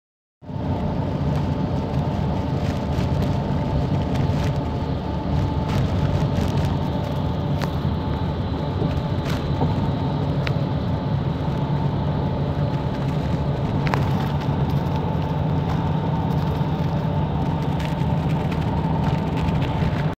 Ponte Rio-Niterói
Sentido Niterói a Rio de Janeiro.
Início do percurso.
22/02/2022
13h25min